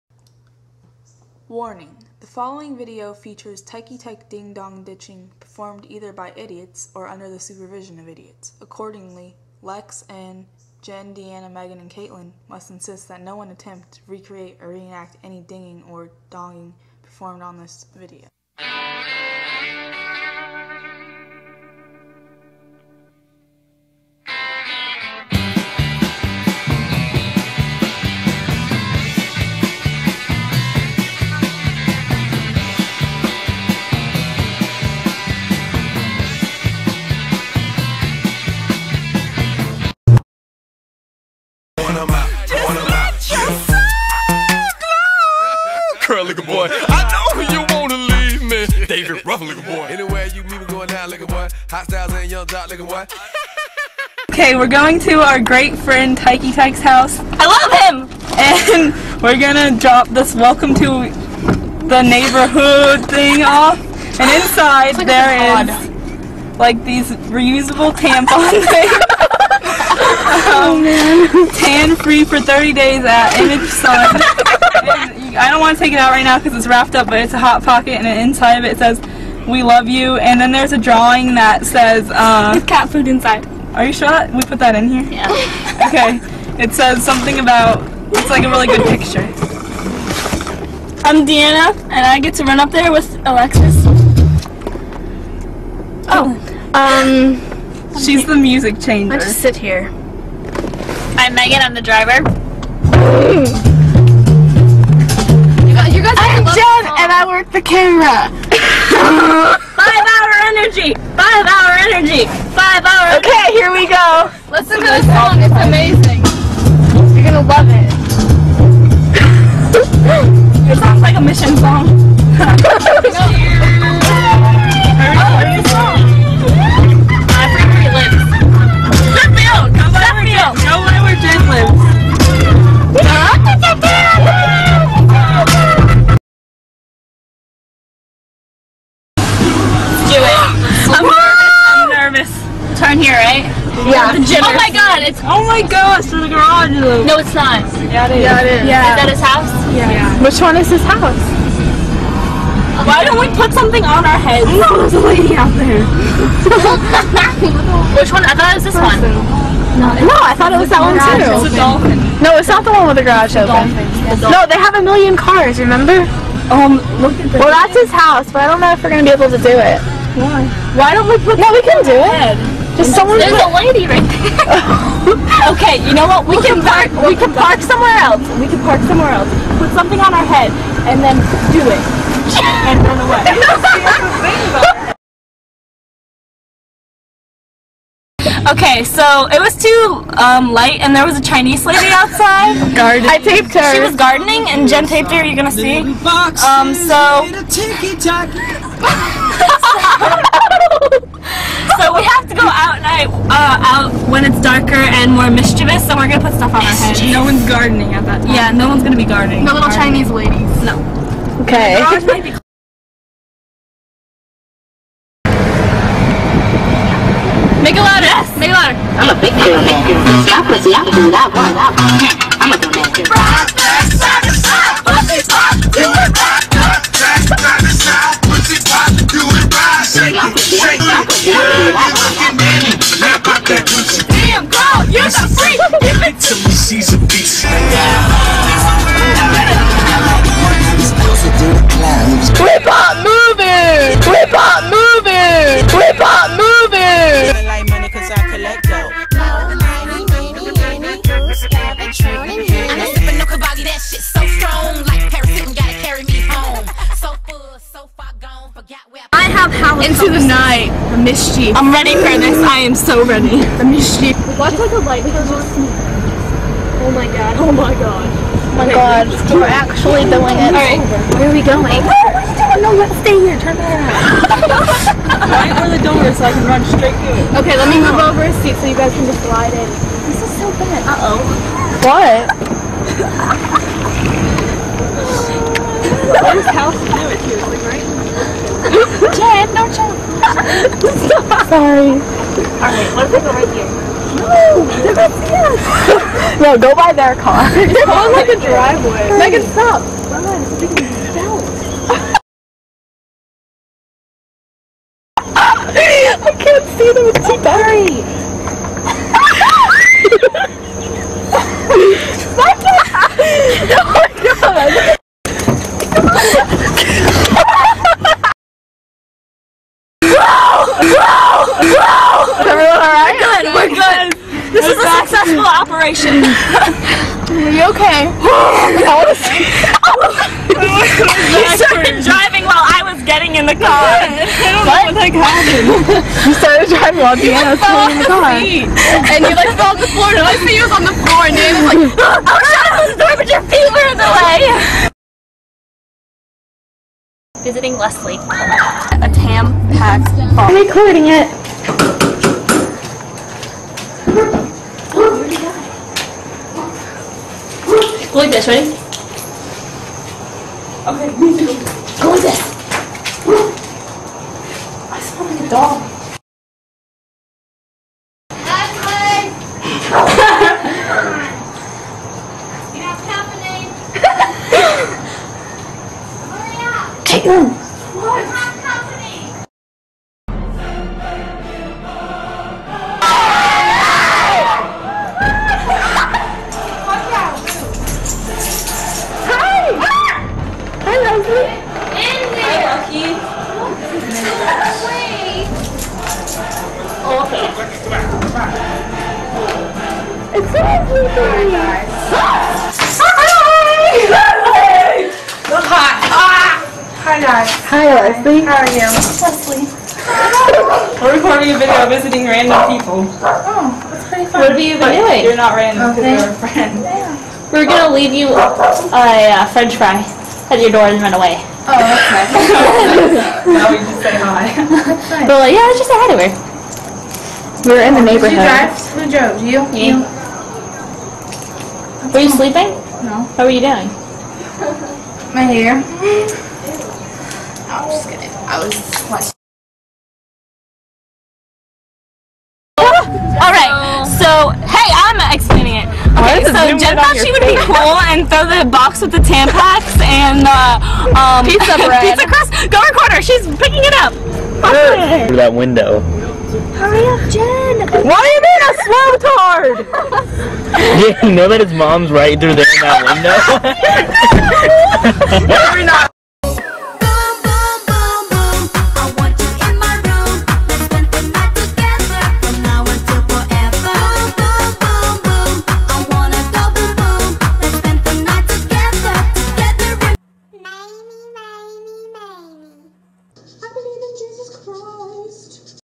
A Night of Mischief
We pulled a ding dong ditch delivery to are bff colin, aka tikey tike.then we do other stufff

this is really bad graphics,but oh well